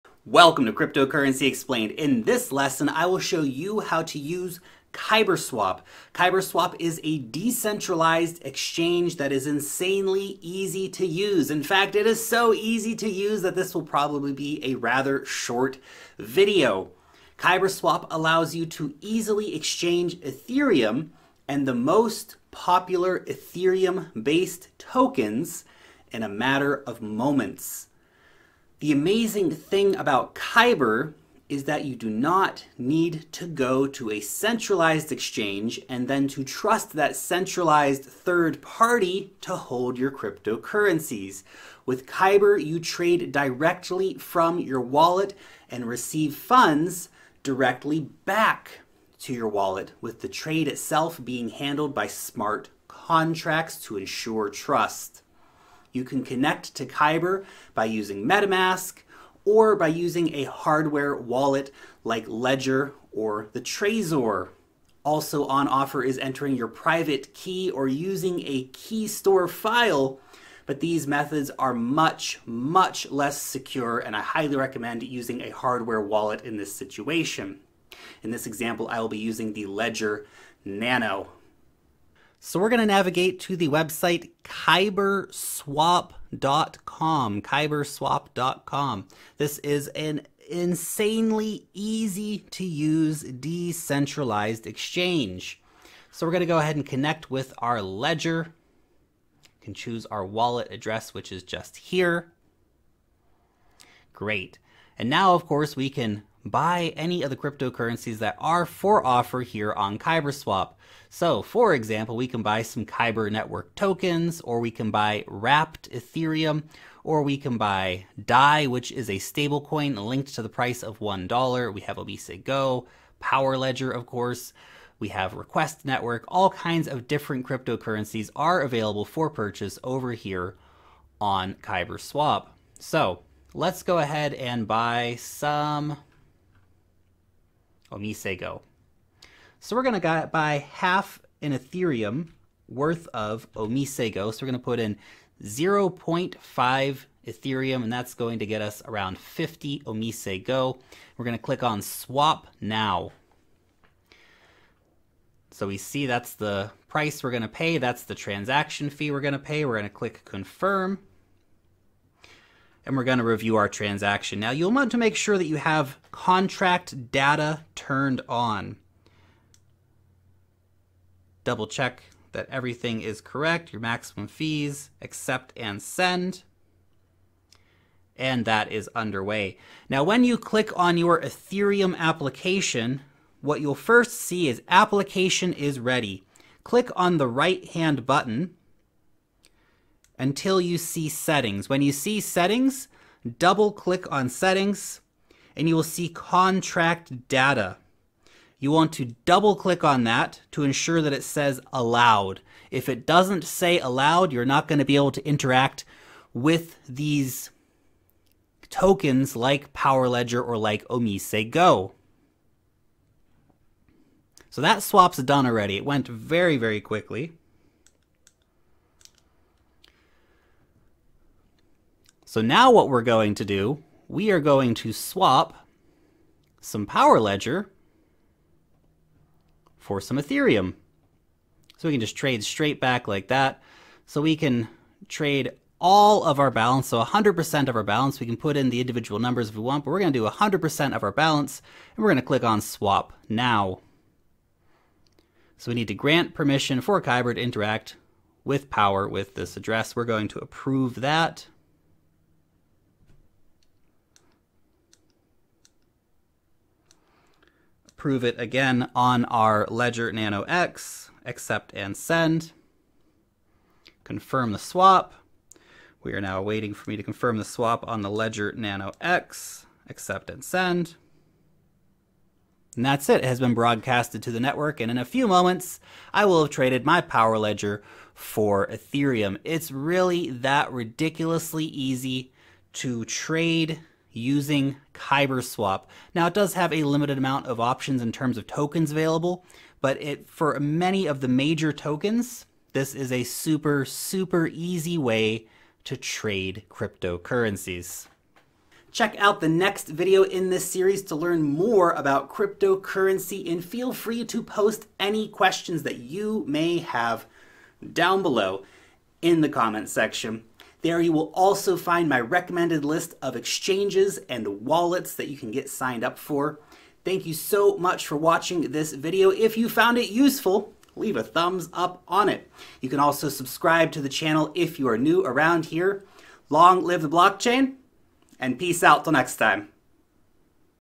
Super Easy Ethereum Token Exchange - Kyber Swap Tutorial - Cryptocurrency Explained - Free Course
Ridiculously easy way to trade Ethereum and Eth based tokens - Kyber Swap

SOCIAL MEDIA  - These are my only accounts, beware of scammers!
D-TUBE d.tube/#!/c/larksongbird